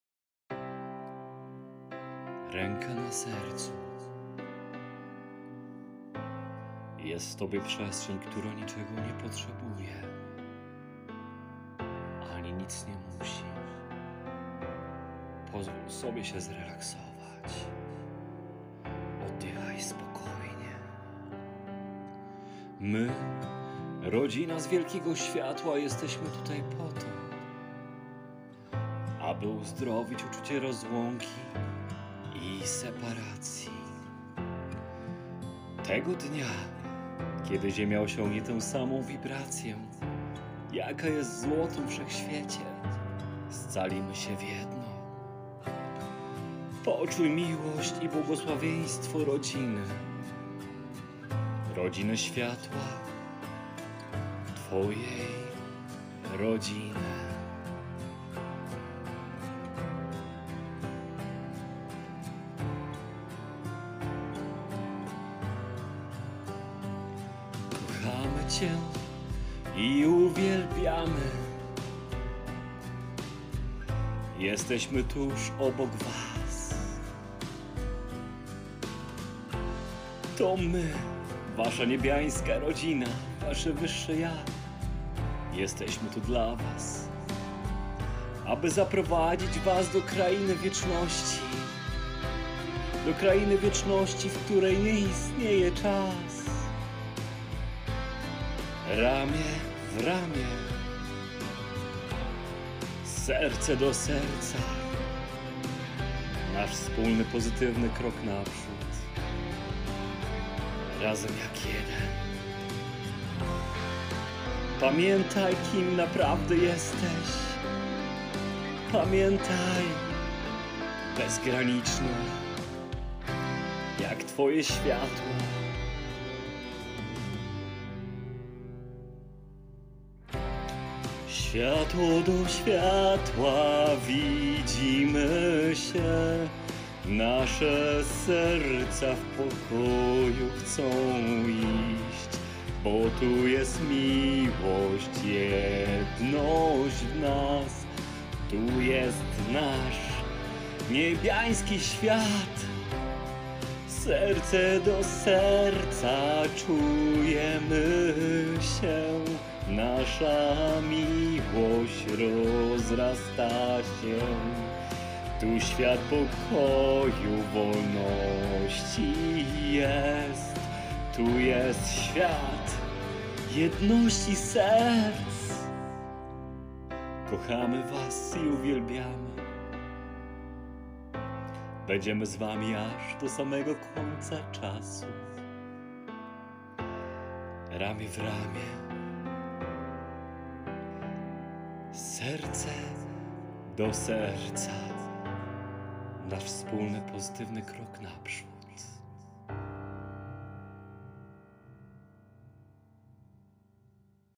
Światło do Światła Serce do Serca
Ręką na Sercu.
Jest w Tobie przestrzeń, która niczego nie potrzebuje, ani nic nie musi. Pozwól sobie się zrelaksować.  Oddychaj spokojnie

My rodzina z Wielkiego Światła jesteśmy tutaj po to, aby uzdrowić uczucie rozłąki i separacji. Tego dnia, kiedy Ziemia osiągnie tę samą wibrację, jaka jest w Złotym Wszechświecie, scalimy się w jedno… Poczuj miłość i błogosławieństwo rodziny, rodziny Światła, twojej rodziny.

Kochamy Cię i uwielbiamy, jesteśmy tuż obok Was. To my, wasza Niebiańska rodzina, wasze Wyższe ja, jesteśmy tu dla was, aby zaprowadzić Was do krainy wieczności, do krainy wieczności, w której nie istnieje czas. Ramię w ramię, Serce do Serca, nasz wspólny pozytywny krok naprzód, razem jak jeden.

Pamiętaj kim naprawdę jesteś, pamiętaj… Bezgraniczny jak twoje Światło.

Światło do Światła widzimy się, nasze Serca w pokoju chcą iść, bo tu jest Miłość, jedności w nas, tu jest nasz Niebiański świat, Serce do Serca czujemy się, nasza miłość rozrasta się, tu świat pokoju wolności jest, tu jest świat jedności Serc!

Kochamy Was i uwielbiamy, będziemy z wami aż do samego końca czasu...

Ramię w ramię, Serce do Serca, nasz wspólny krok naprzód.